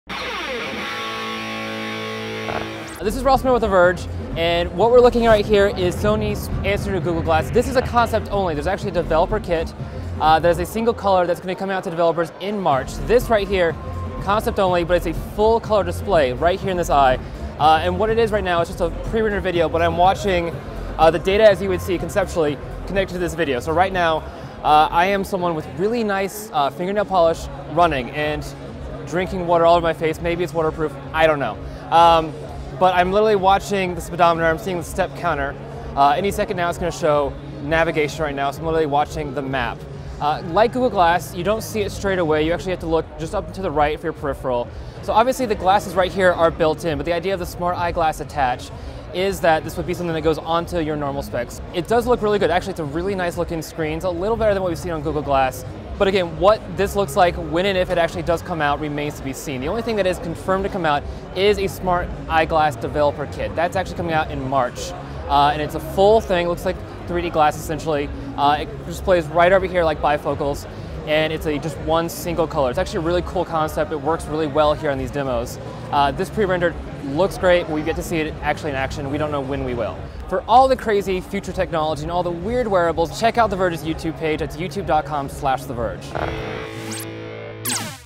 This is Sony's answer to Google Glass — CES 2015
Sony's SmartEyeglass Developer Edition is just a couple of months away from release — full-frame smart glasses that projects green-hued games and apps directly in front of you.

We try out a working prototype as well as a very Google Glass-like concept that displays a full color screen just outside of peripheral — and would theoretically attach to your own glasses.